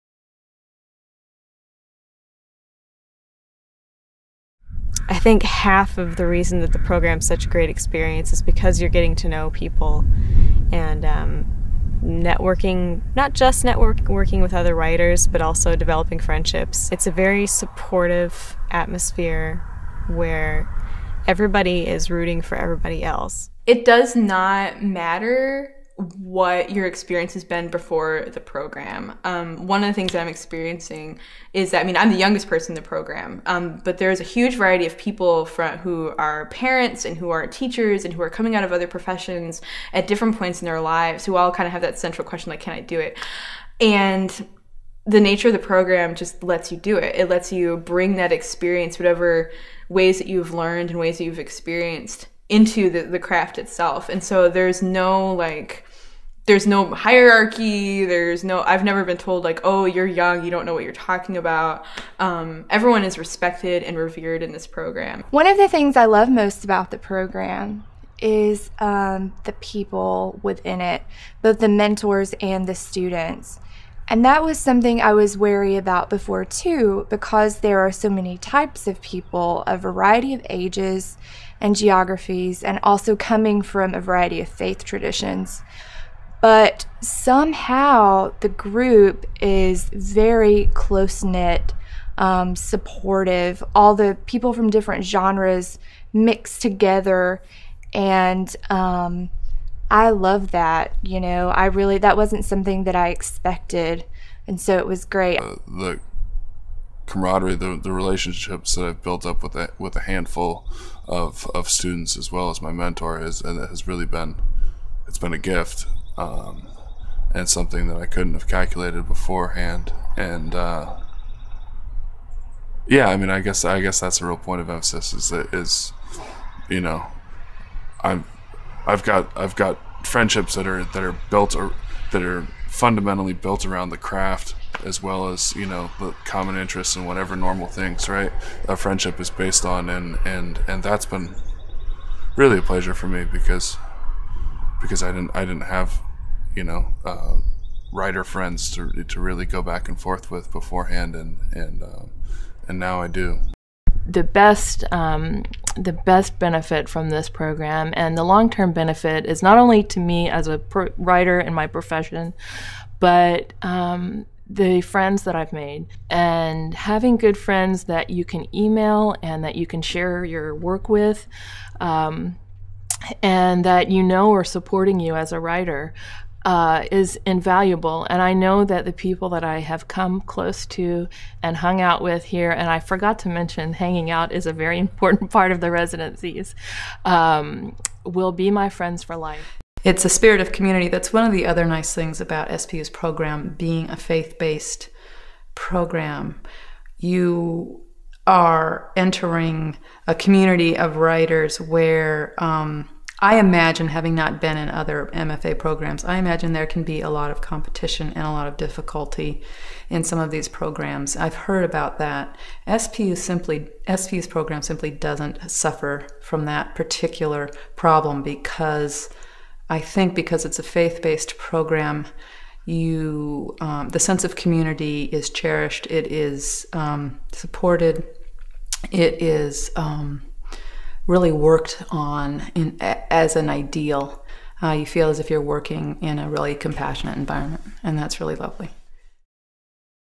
A Supportive Community - SPU's Master of Fine Arts Program
Students in Seattle Pacific University's Master of Fine Arts program discuss the sense of community that develops in the program.